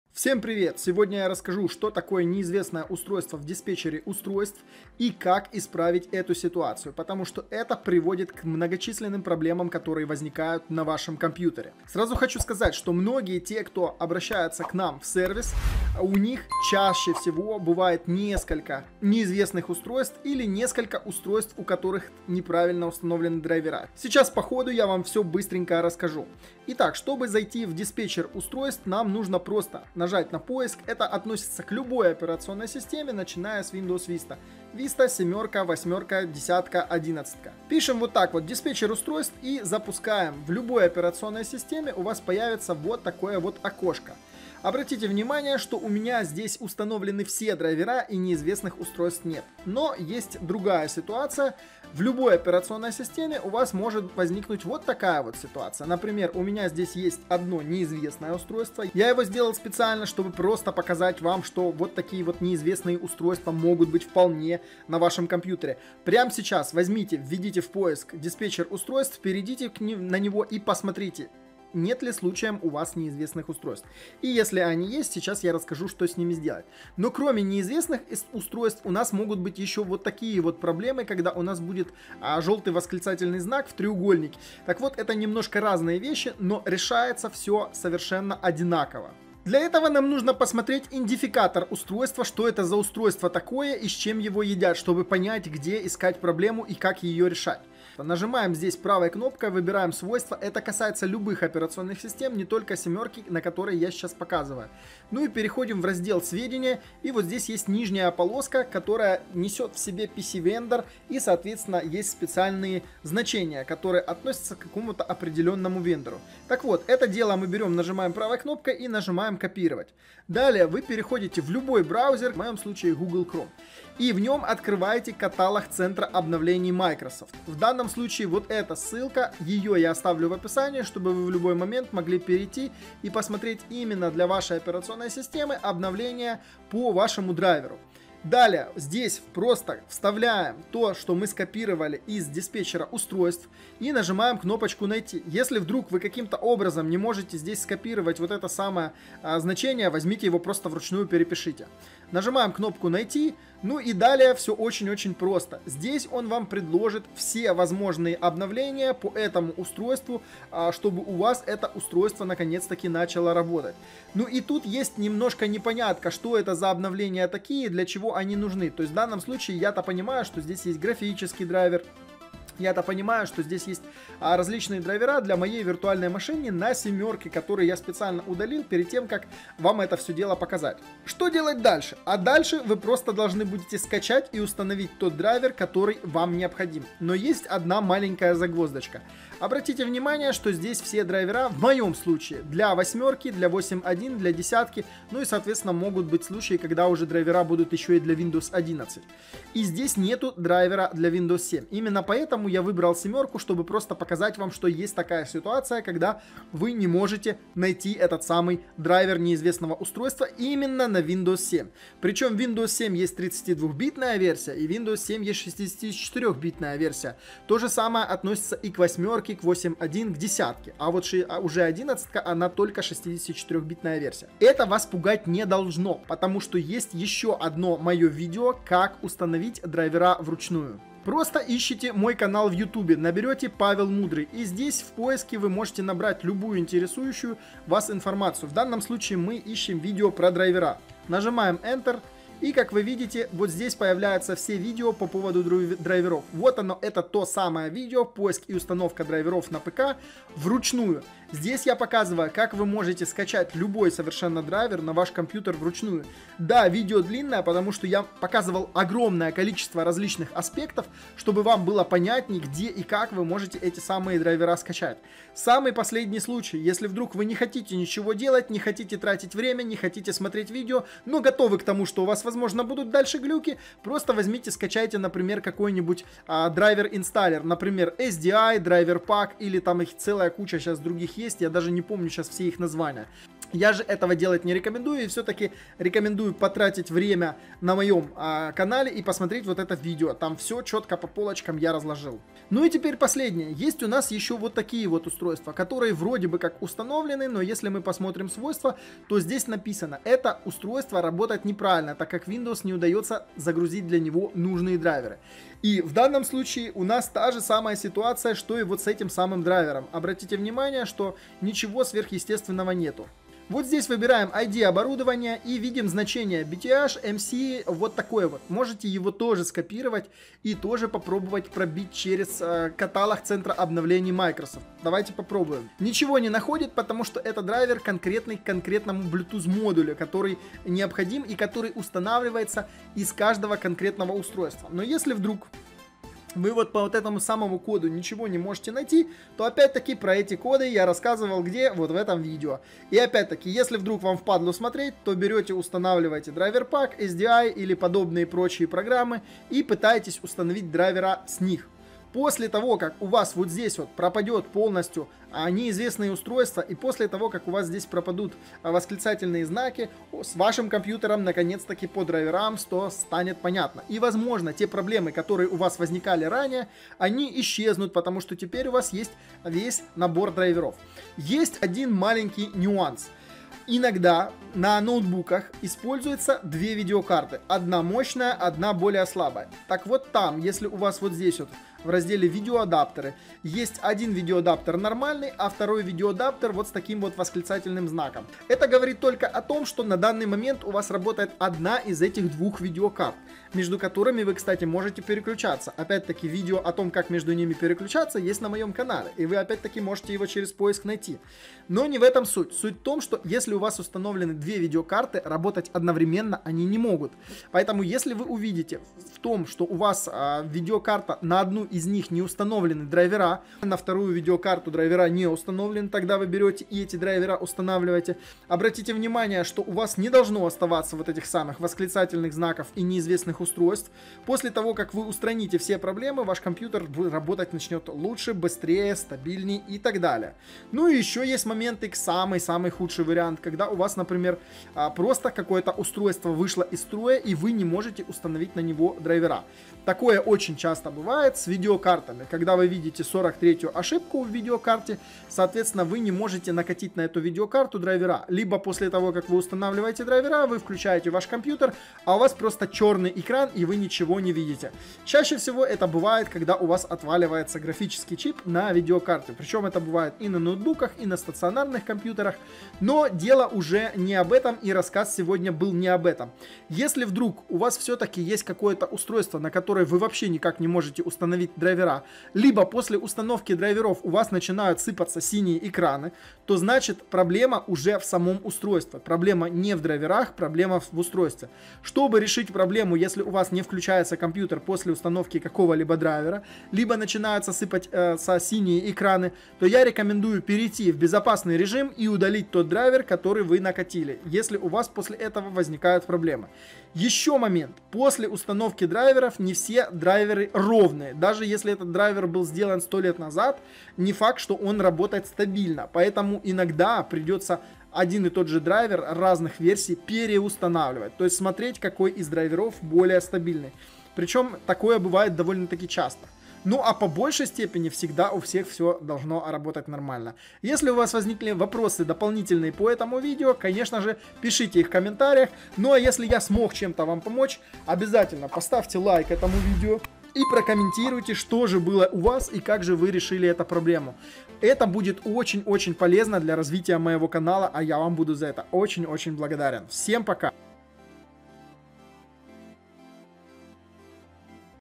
❓ Неизвестное устройство в Windows. Как решить проблему?
Прямо сейчас проверь свой Диспетчер устройств на наличие Неизвестных устройств или устройств с сбойными драйверами. Что бы исправить все неизвестные устройства смотри мое видео!

Ключи на Windows 11 Pro и 10 Pro по выгодной цене